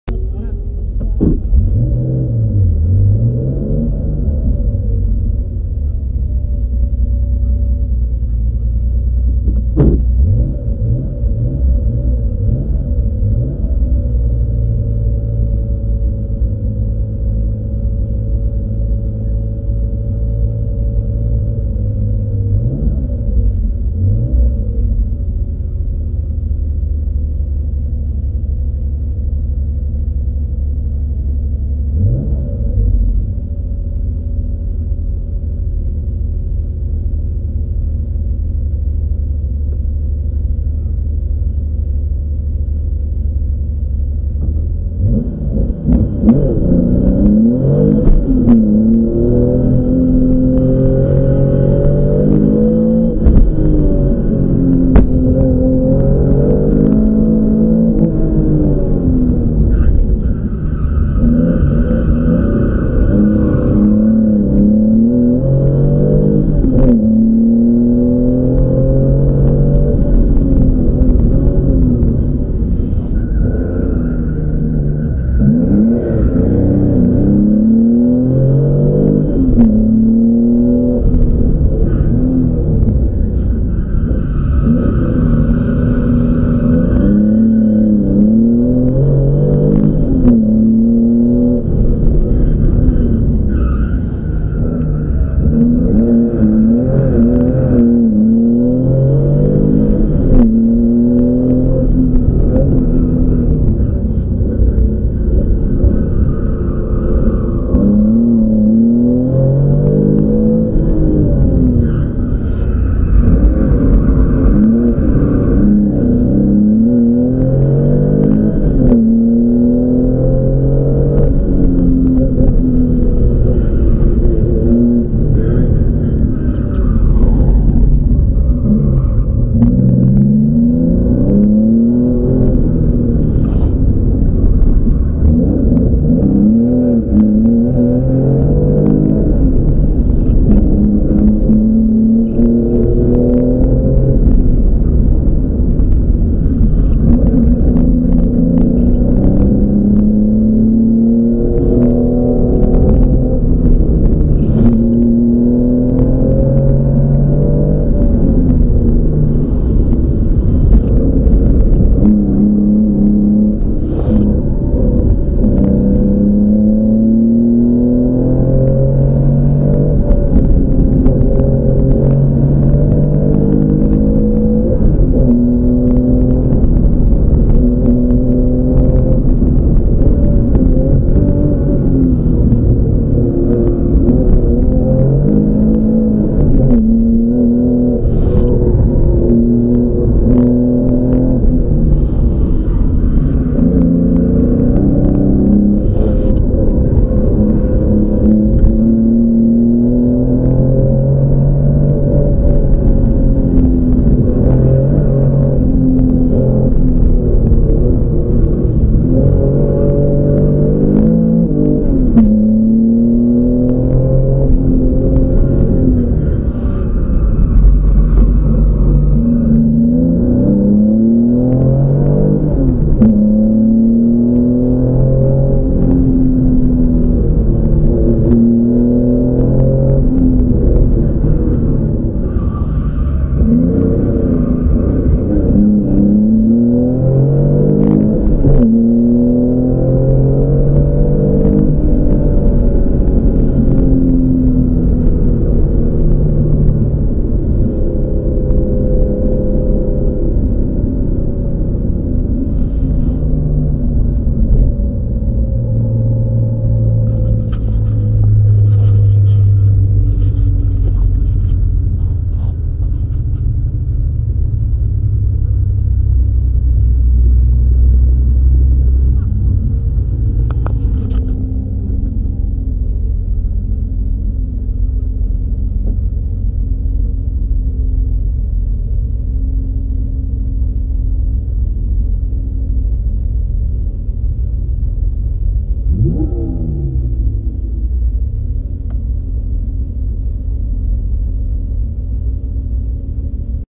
race 1 omalos 28/08/2011 in car
In car cam of the first race run, Omalos hillclimb, Hania Crete.
Several line mistakes on very slippery tarmac with no LSD but it was fun and got me a first and a second class podium places on my first ever Greek championship race. Time 3:14 on this run and 3:11 on the second.